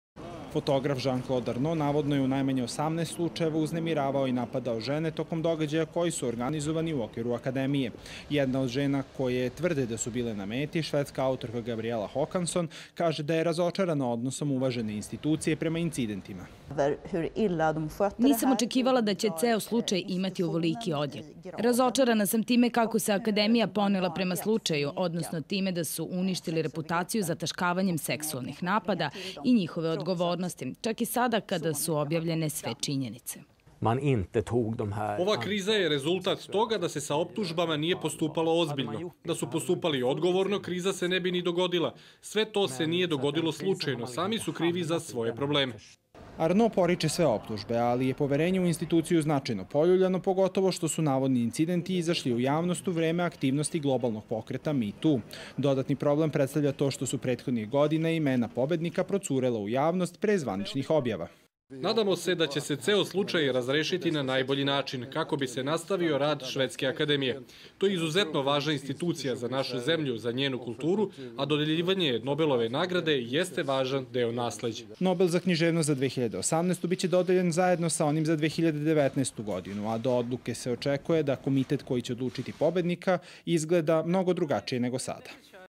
Ove godine bez Nobelove nagrade za književnost
Nobelova fondacija saopštila je danas da ove godine neće biti dodeljena Nobelova nagrada za književnost zbog skandala vezanom za optužbe za seksualno uznemiravanje u vrhu Švedske akademije, zbog čega je više članova napustilo ovu instituciju.